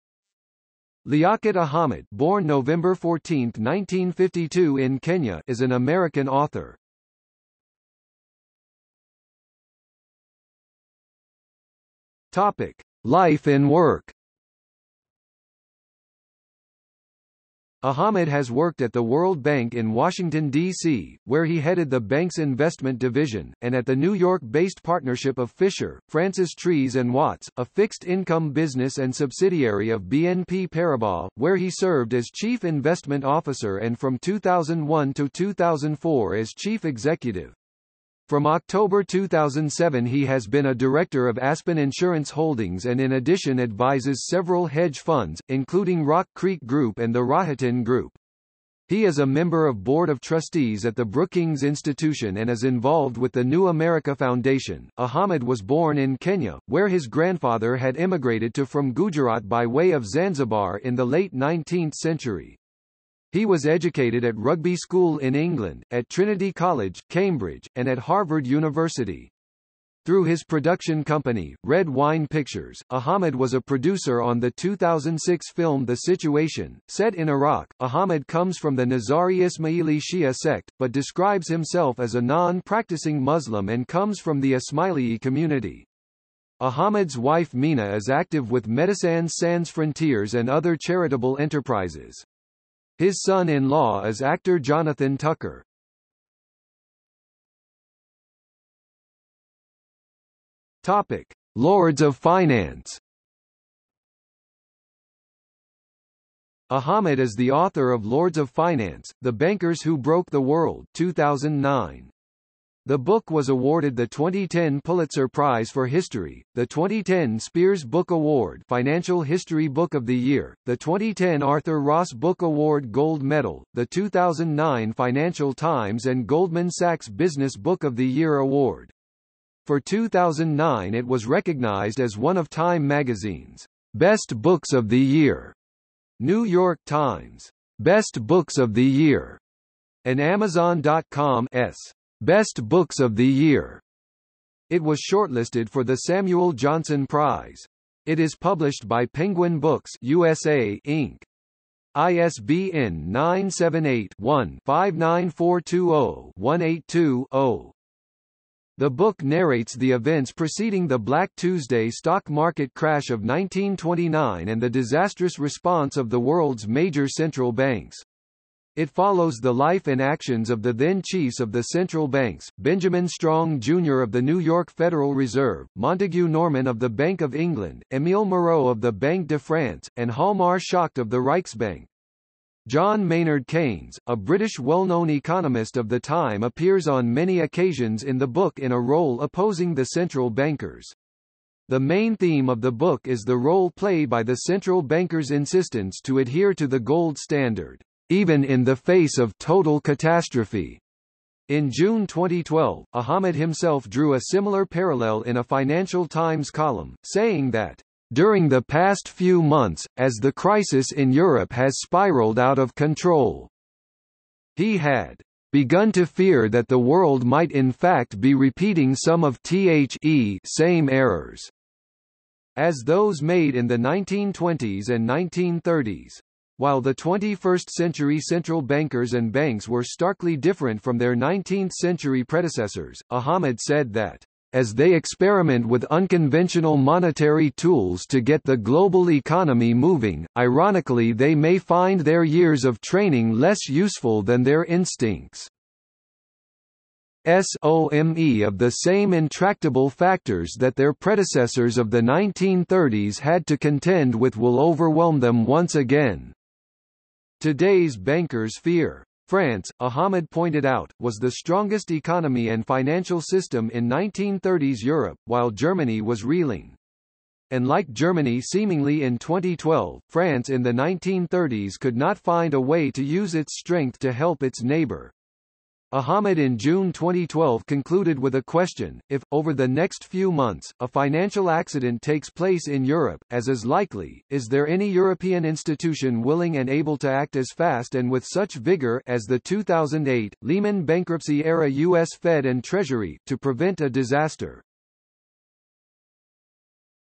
Liaquat Ahamed | Wikipedia audio article
00:00:08 1 Life and work
00:01:37 2 iLords of Finance/i

- Socrates

SUMMARY
Liaquat Ahamed (born November 14, 1952 in Kenya) is an American author.